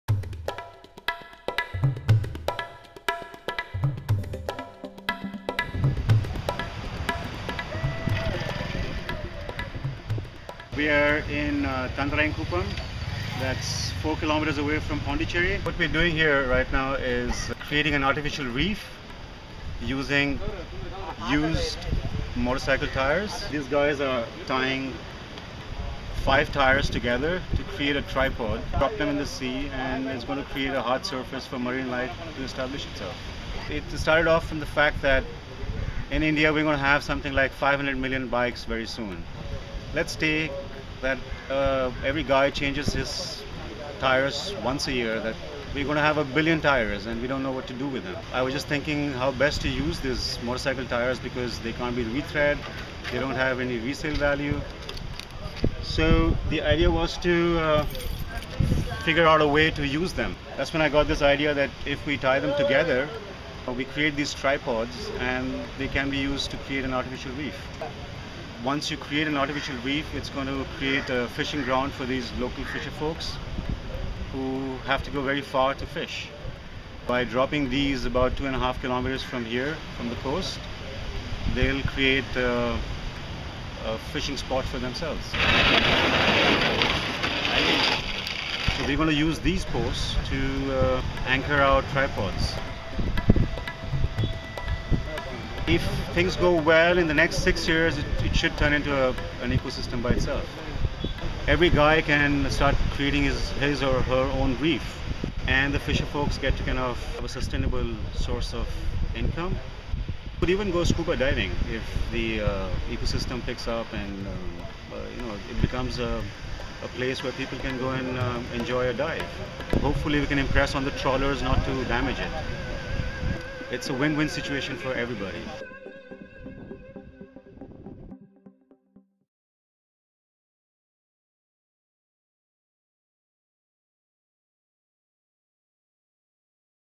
motorbike tyre tripod
Dev in Pondicherry Tamil Nadu is using old and otherwise useless motorbike tyres to make the foundation off an off shore reef.
Shot with a flipcam